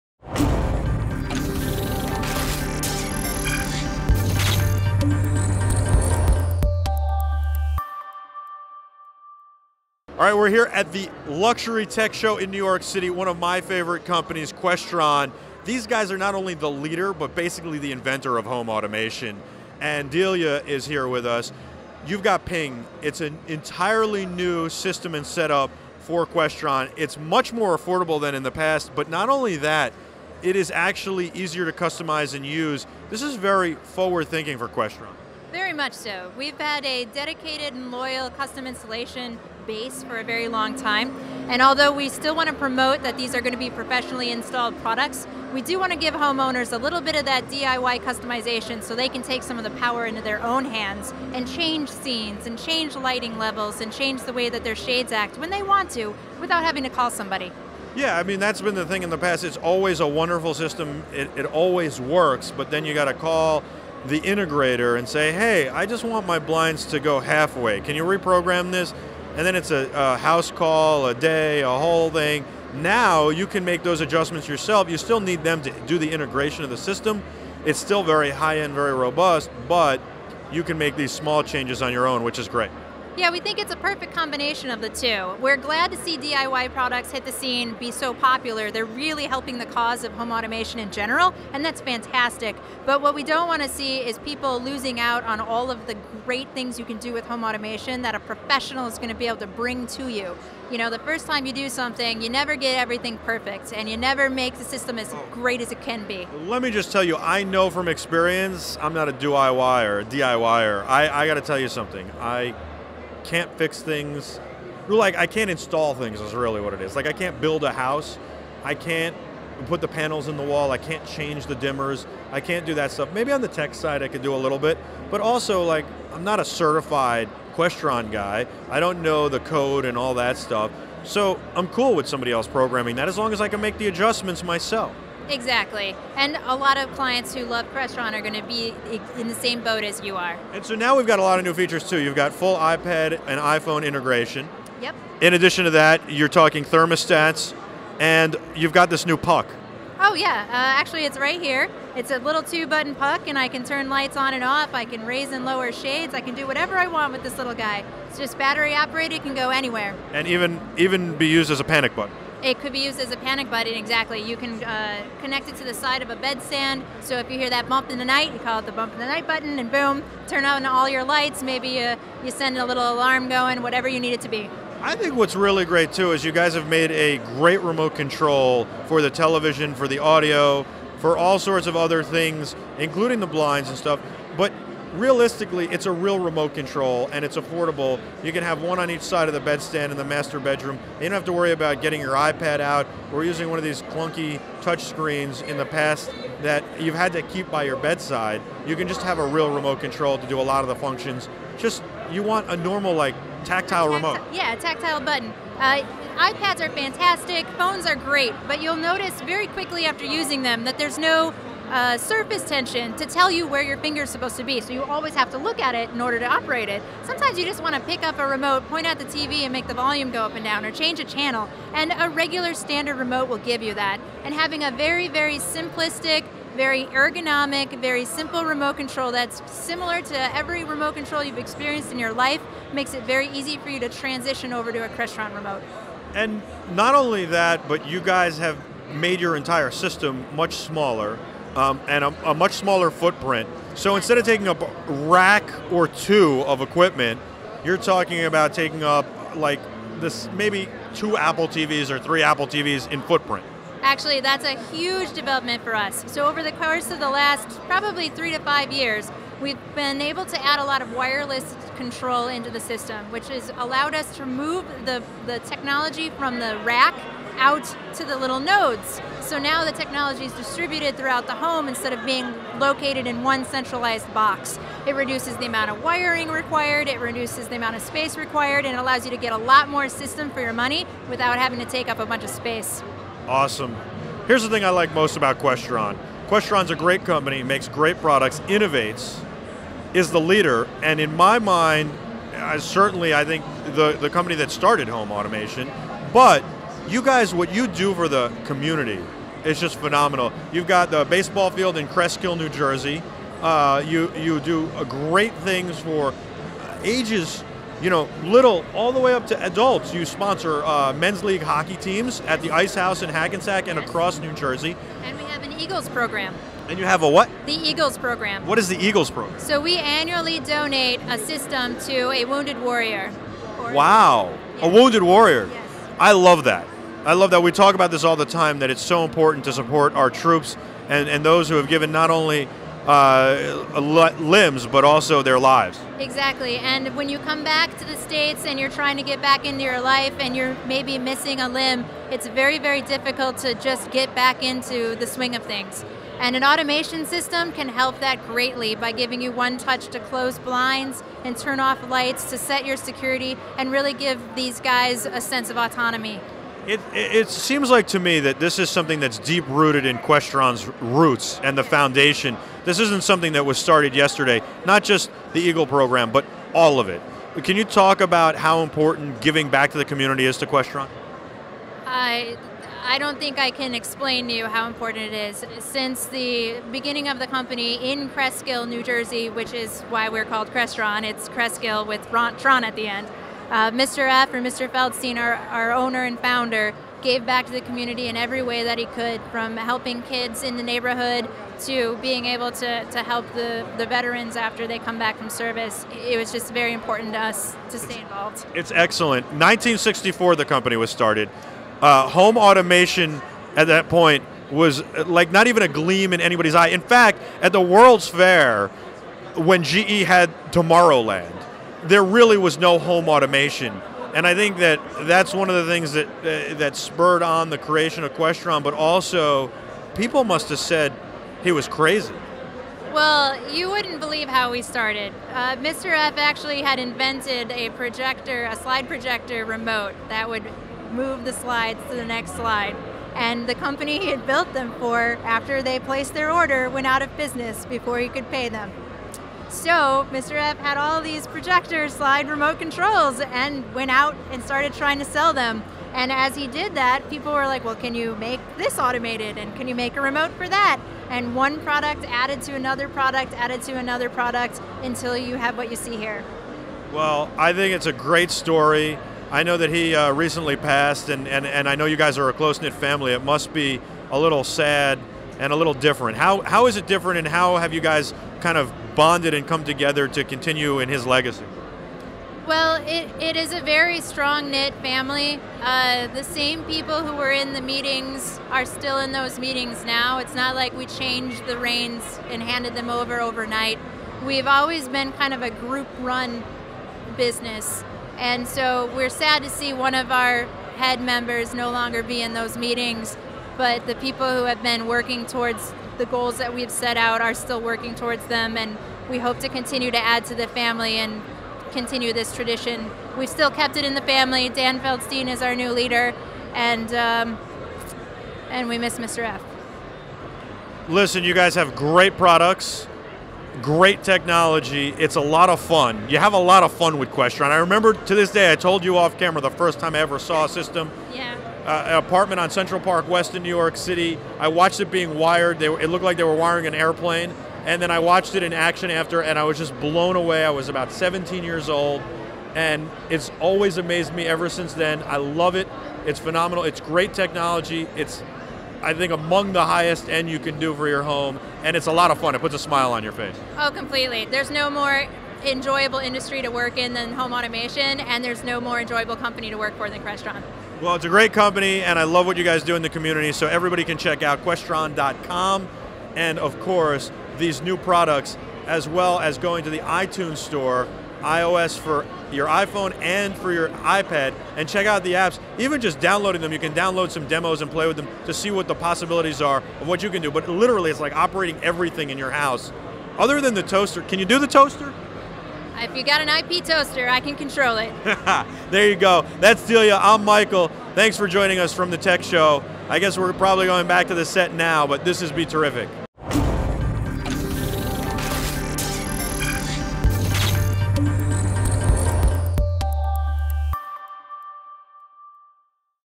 Crestron Pyng at the NY Luxury Tech Show!
Crestron's Delia Hansen joins us at the Luxury Tech Show in NYC to show us the brand new Crestron Pyng home automation system!